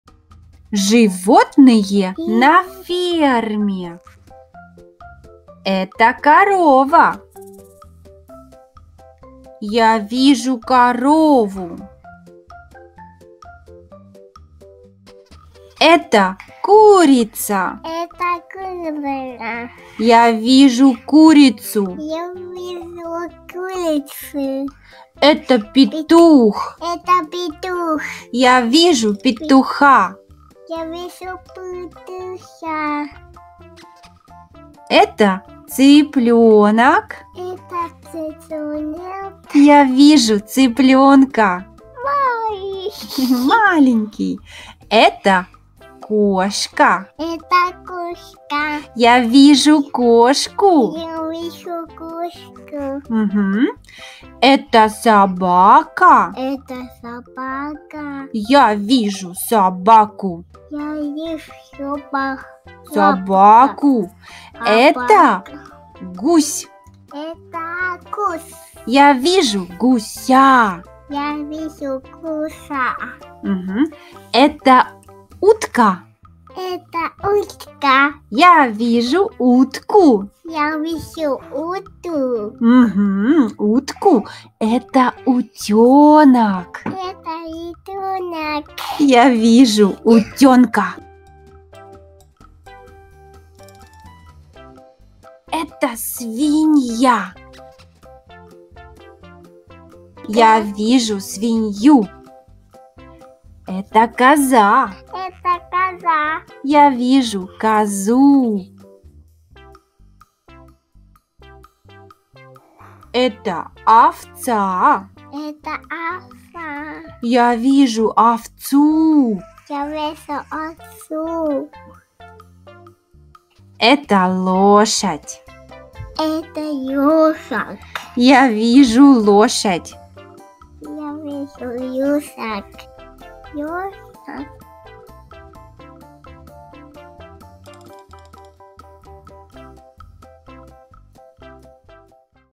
Животные. Listen how Accusative case works in practice and how kids learn Russian in a natural way.
Learn animals in Russian and listen how endings of the words change in Accusative case.

Would you like to learn Russian online with a professional native speaker in a small group or individually ?